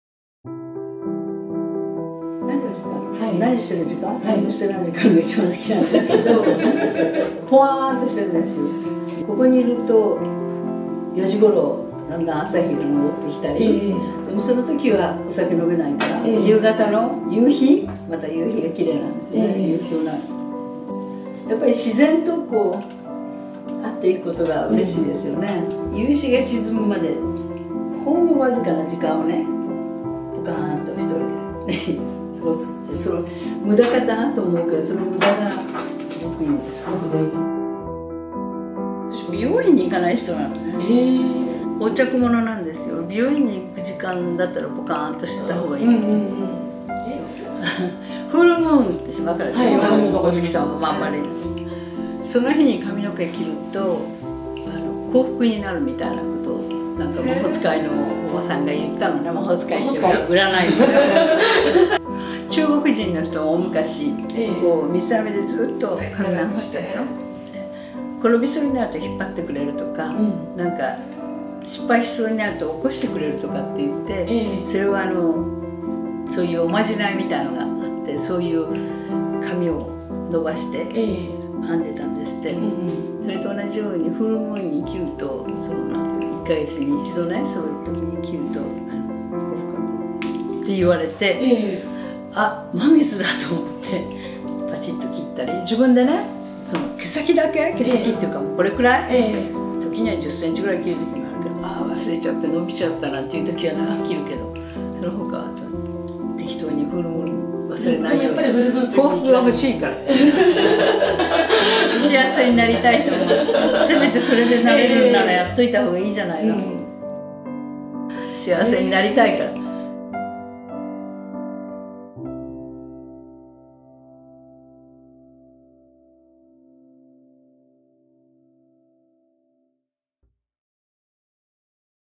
デザイナー島田順子さん、自分スタイルの秘訣は？「髪の毛は満月の日にセルフカット」｜Precious.jp
パリコレクション35周年を迎えた島田順子さん。
75歳のいまもかこよく、愛らしく、全女性の憧れの存在です。島田順子さんらしさの特徴でもあるヘアスタイルやファッションについて教えていただきました。

インタビュー記事はこちら。